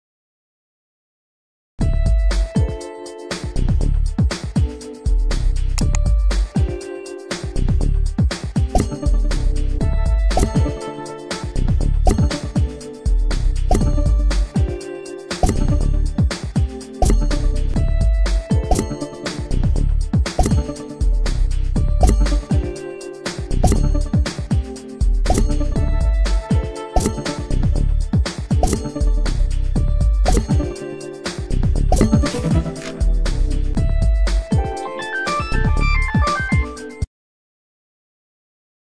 AI solve USA Today the Daily Word Search : 8/11/2023
Watch AI solve USA Today the Daily WordSearch !